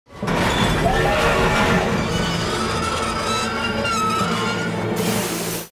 Dinosaur trains emergency Brake down stop(read description)
Just Incase he brakes down he has to take a big emergency stop

@Smudgy Bear@PrimalyVoid2025@MickyleG345@springdudeofgod7795@Fuzzy_1304@peteyhernandez4942@Gachatuber2003@tamezlocomotivestudios6493